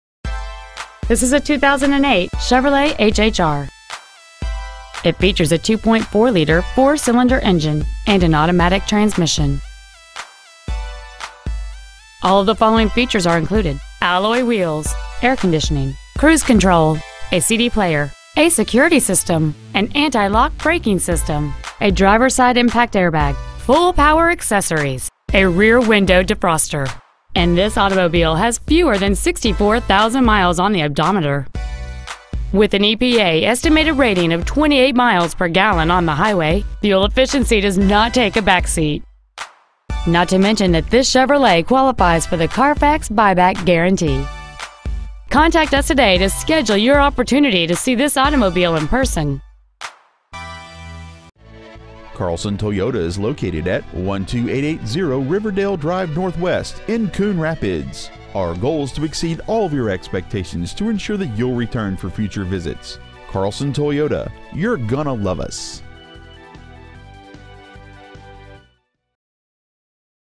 2008 Chevrolet HHR Coon Rapids MN 55448
Year : 2008
 Make : Chevrolet
 Model : HHR
 Engine : 2.4L 4 Cyl. Fuel Injected
 Trans . : Automatic
 Exterior : Sandstone Metallic
 Miles : 63,869

 Stock : 3144L